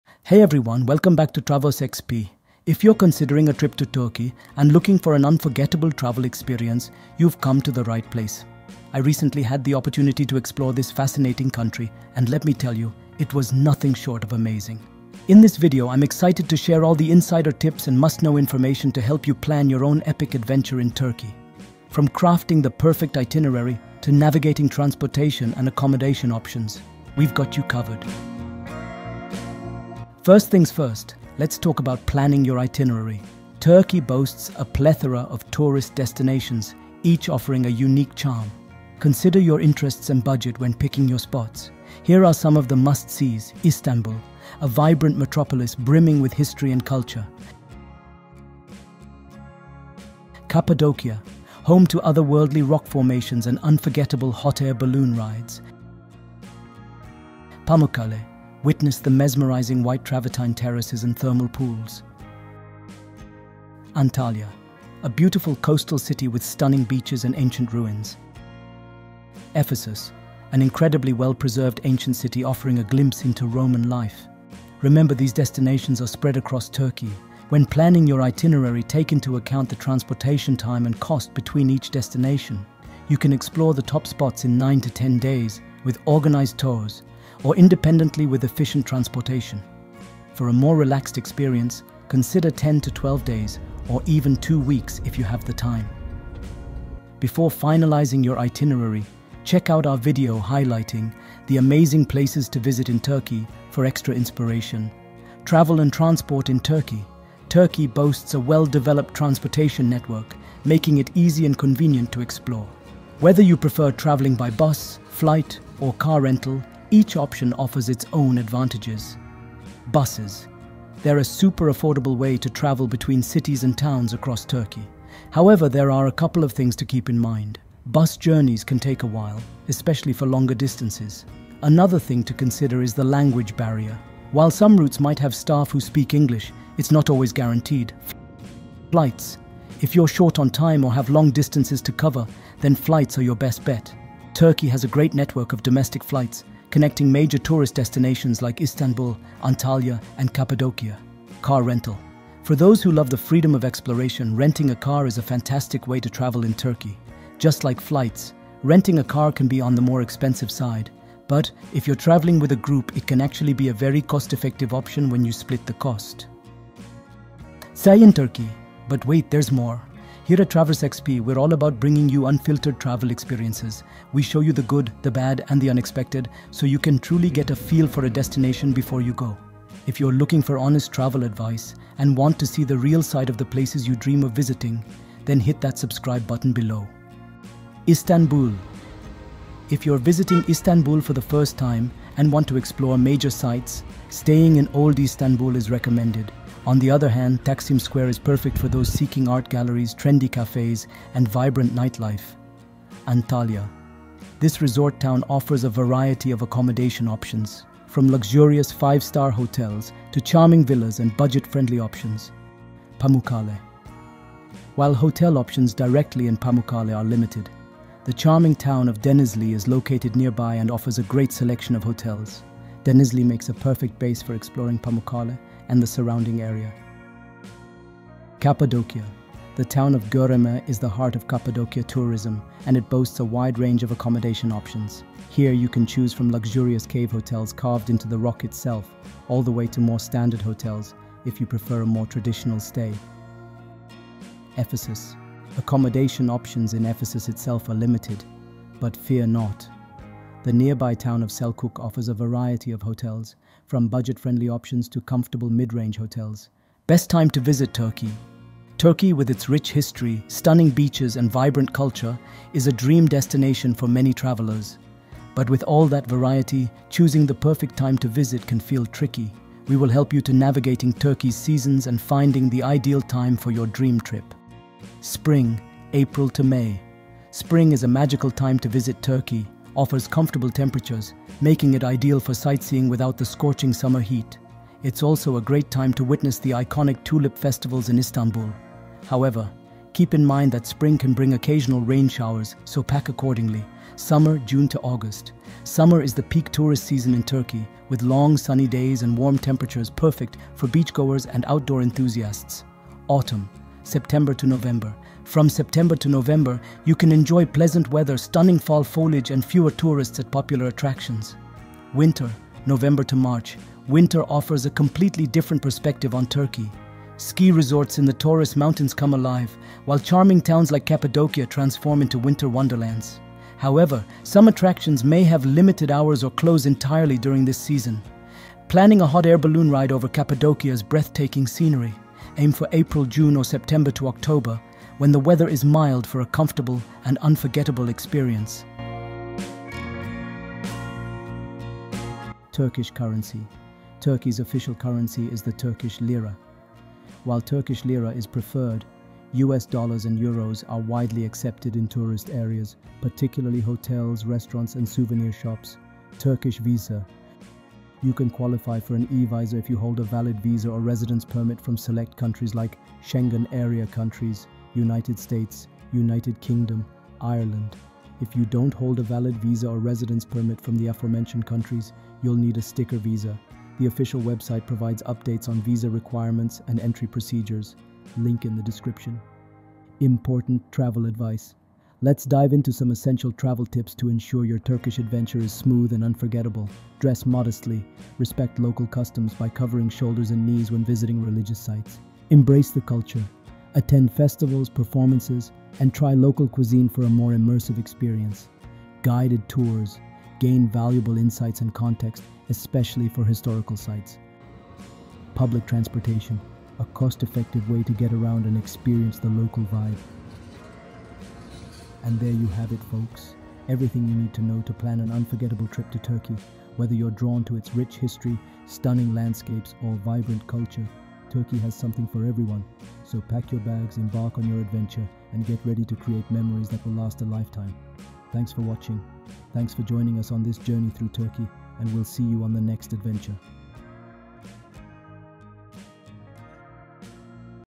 Turkey Travel Guide 2024 | Best Time To Visit Turkey | Turkey Tour Guide 2024
You can book your tickets here with a good discount at GetYourGuide.com

0:00 Intro
0:32 Turkey Tour Itinerary
1:46 Transport In Turkey
2:48 Accommodations In Turkey
4:17 Best Time To Visit Turkey
5:28 Best Time for Hot Air Balloon Ride
5:42 Turkey Currency
5:56 Turkey Visa for Travellers
6:20 Turkey Travel Advice

About Video
Discover the beauty and wonder of Turkey with our comprehensive "Turkey Travel Guide 2024" video! 🇹🇷✨

Join us as we delve into all the essential aspects of planning your dream trip to Turkey. From the best time to visit this mesmerizing country to insider tips on currency, accommodations, transportation, and more, we've got you covered every step of the way.

In this video, we explore:

The optimal seasons to visit Turkey for the most memorable experiences.
A breakdown of the Turkish currency and the most convenient ways to handle your finances while traveling.
A variety of hotel options to suit every budget and preference, ensuring a comfortable and enjoyable stay.
Transport options for touring Turkey, including buses, trains, and other convenient modes of travel to explore the country's diverse landscapes and attractions.
Whether you're a seasoned traveler or embarking on your first adventure abroad, our "Turkey Travel Guide 2024" provides invaluable insights and recommendations to help you plan the perfect trip to this enchanting destination.

Don't miss out on the opportunity to uncover the treasures of Turkey and create unforgettable memories that will last a lifetime. Watch our video now and start planning your Turkish adventure today! 🌍✈️